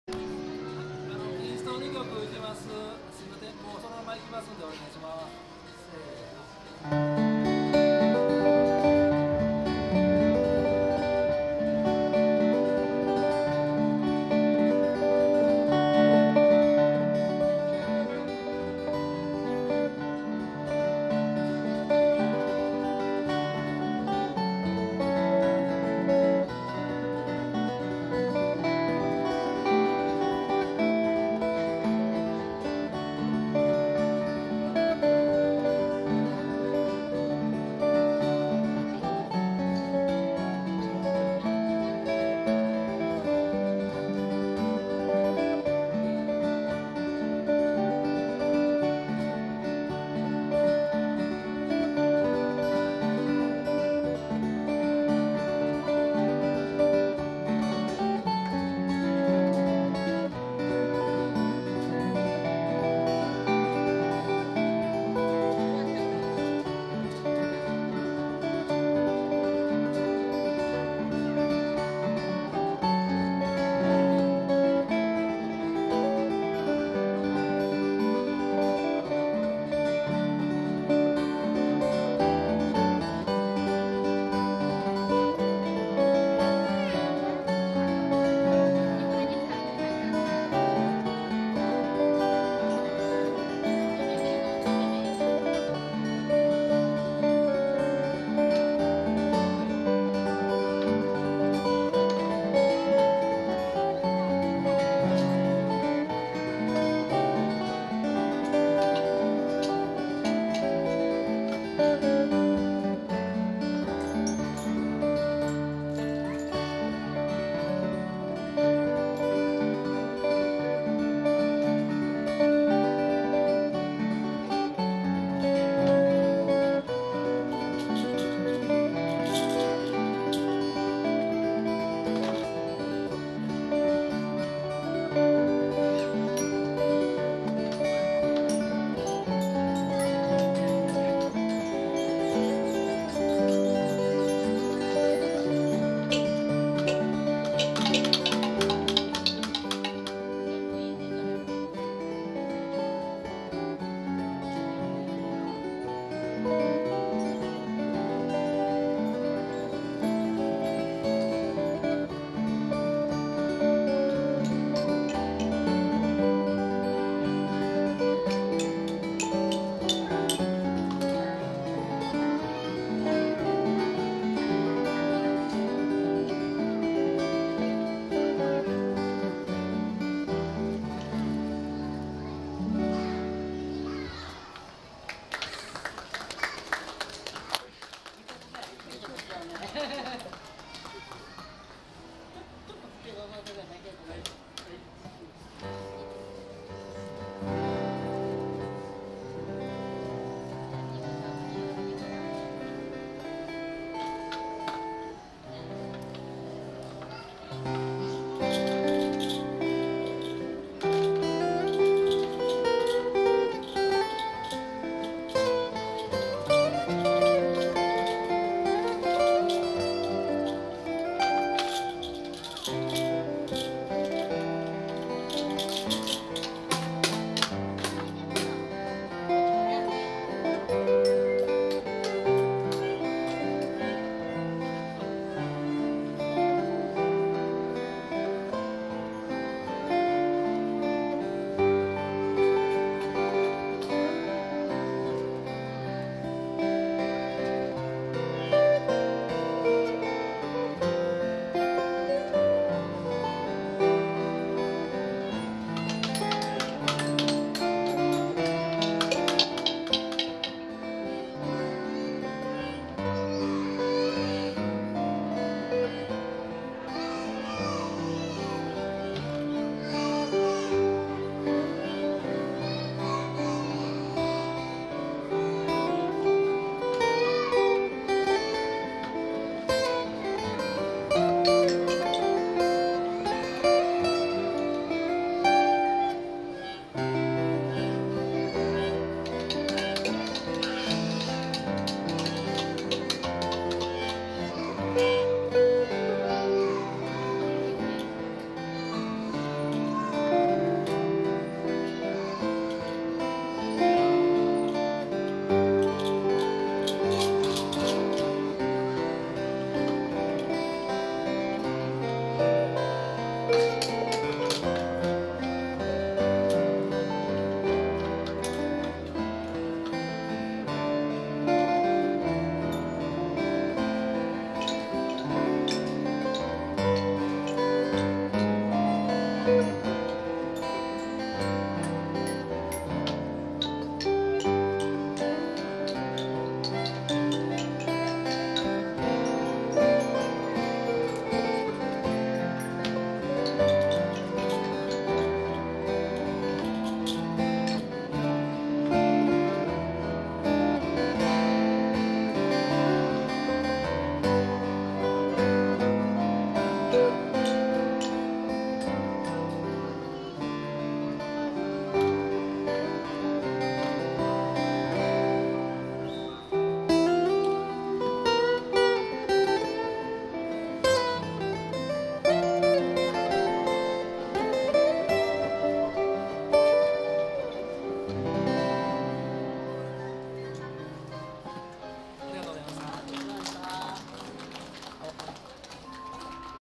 K＆M 第4回アコパラ 島村楽器泉南店 店大会
第4回アコパラ出場「K＆M」のライブ映像です。
2月17日にイオンモールりんくう泉南店で開催された第4回アコパラ店大会のライブ映像です。